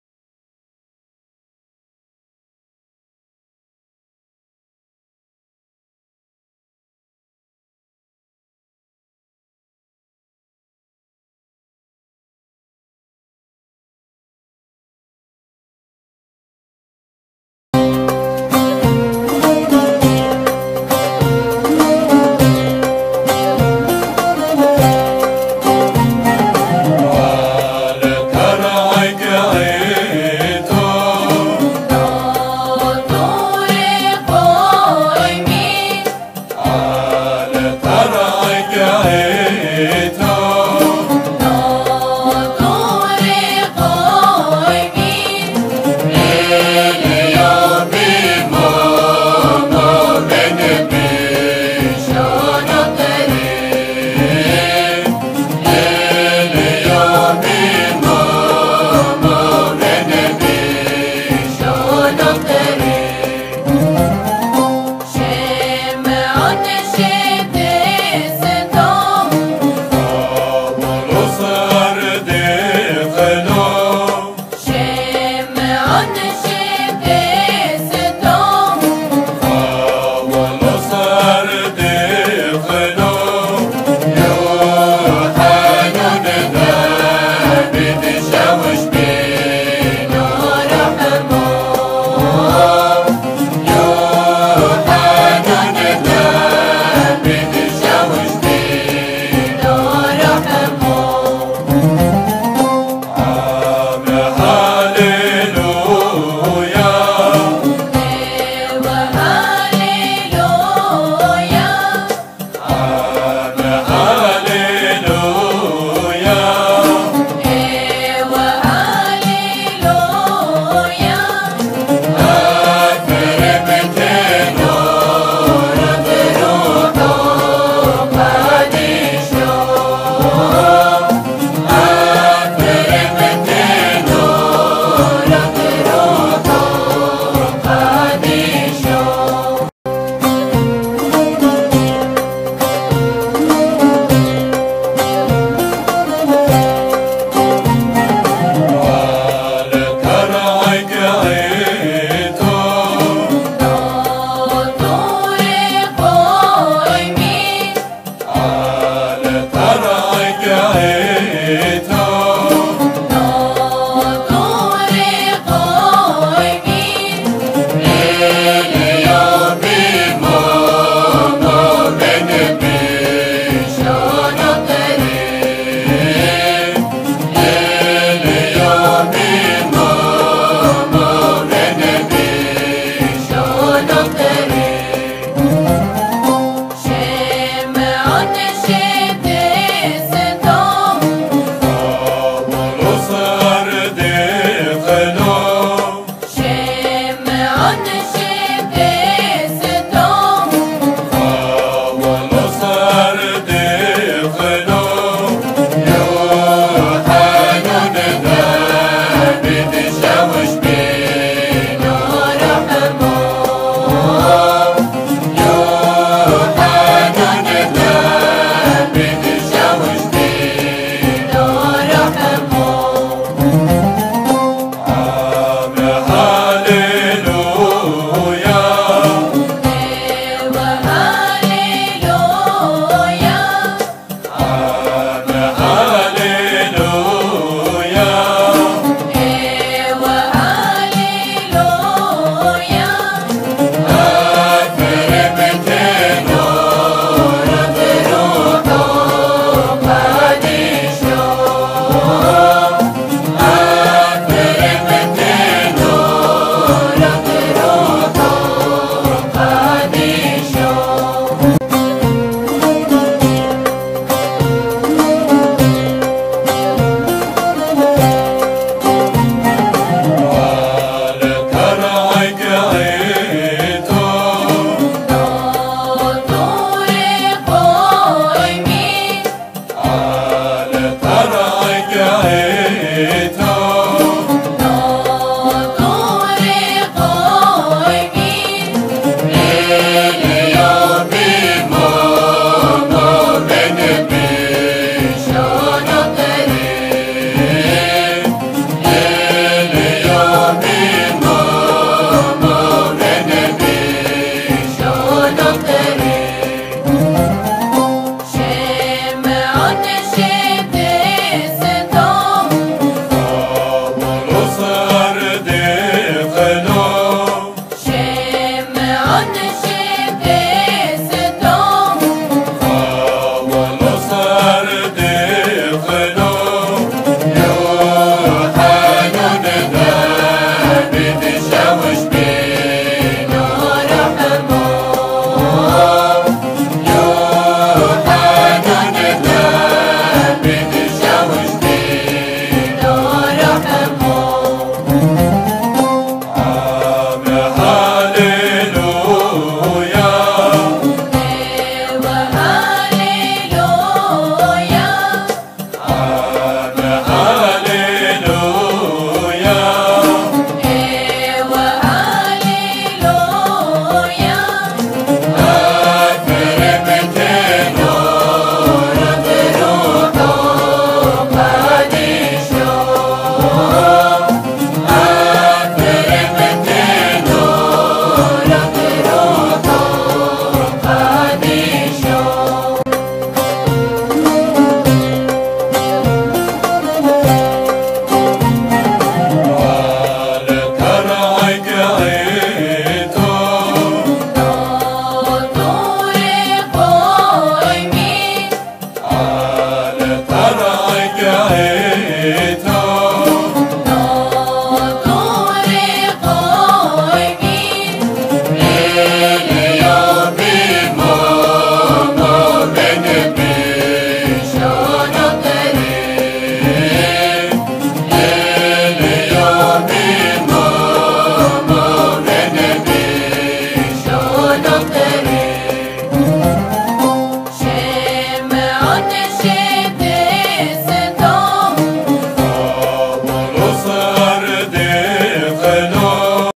Mor Augin Harsewinkel
Weihung von Messdienerinnen&Messdienern am 20 November 2011 in der syrisch orthodoxen Mor Augin Kirche Harsewinkel durch seine Eminenz Mor Yulius Hanna Aydin